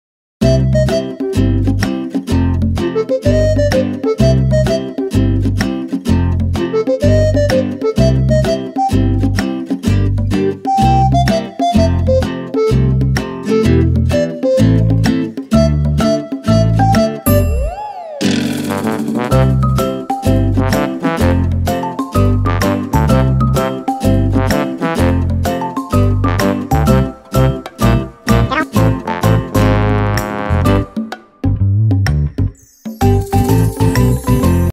Chonky munchkin kitty transformation :D
Just another daily short video of my kitten Darong :D she became a little chonky cat but still freaking darn adorable